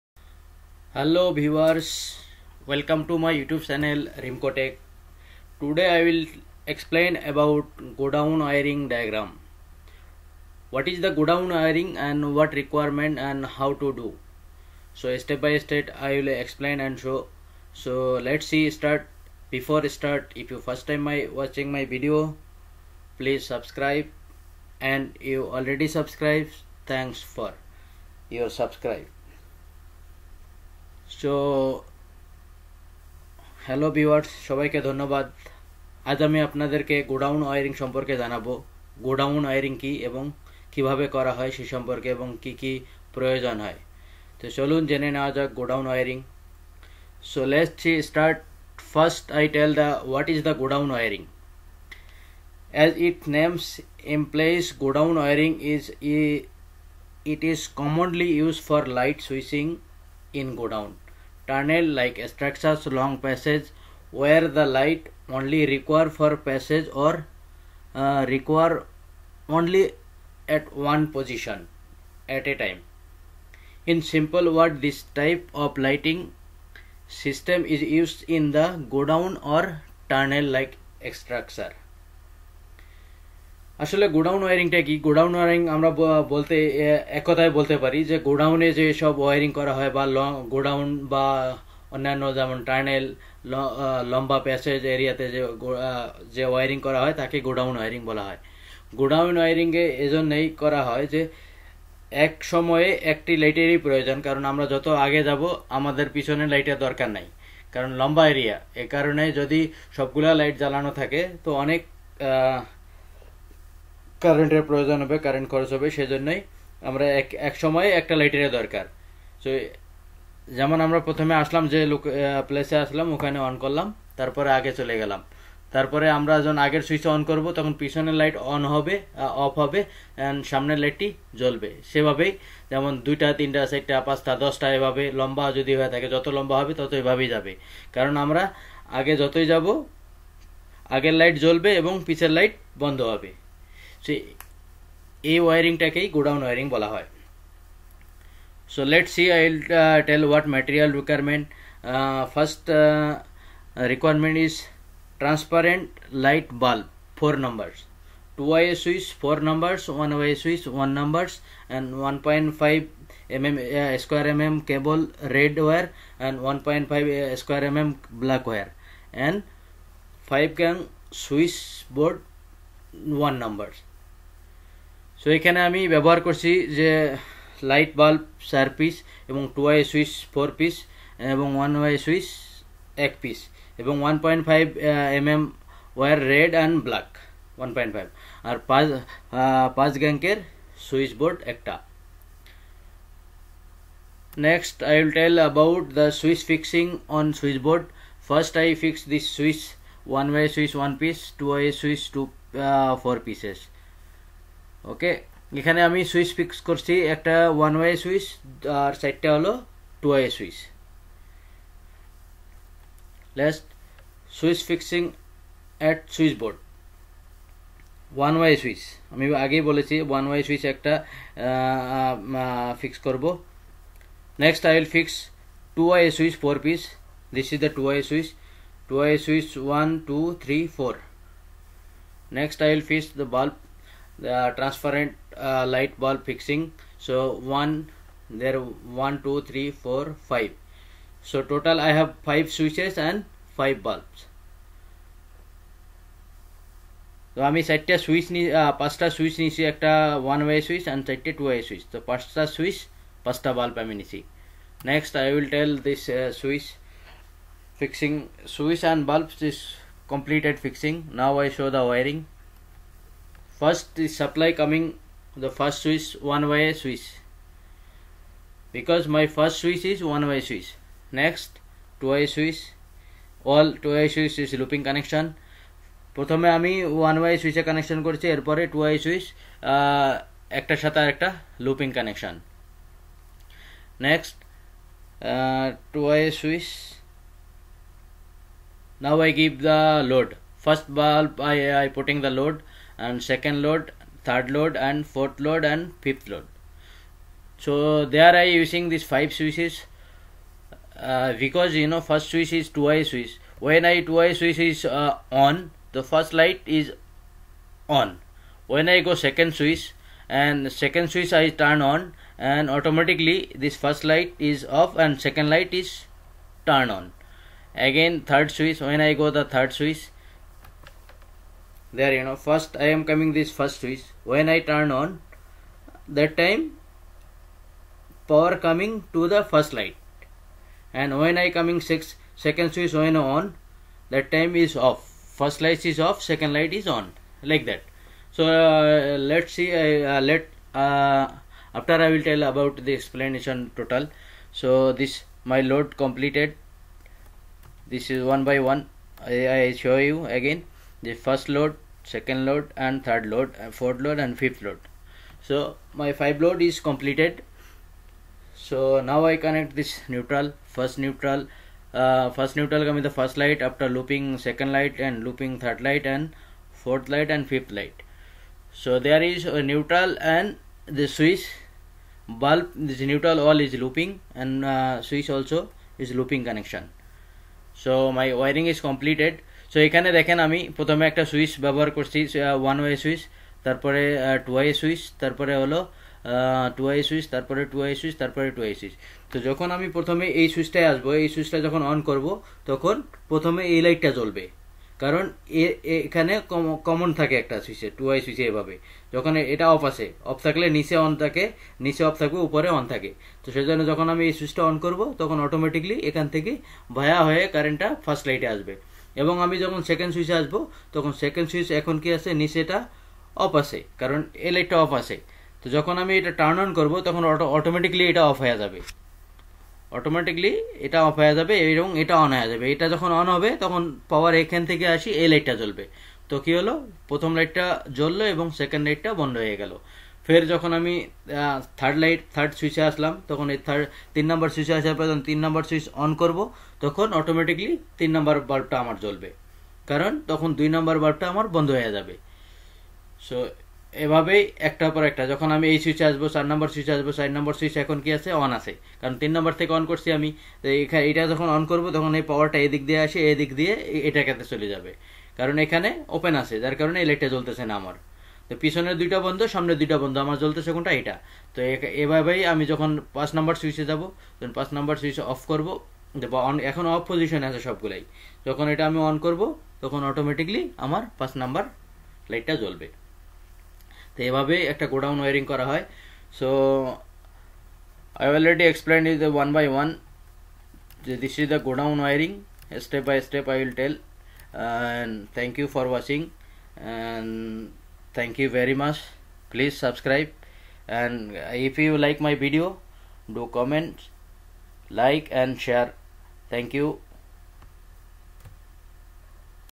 godown wiring circuit & connection explained ।। গোডাউন ওয়্যারিং
remco tech what is the godown wiring meanings how to do godown wiring step by step explained english and bangla show circuit diagram connection switch and bulb please if you like my video subscribe remco tech গোডাউন ওয়্যারিং ।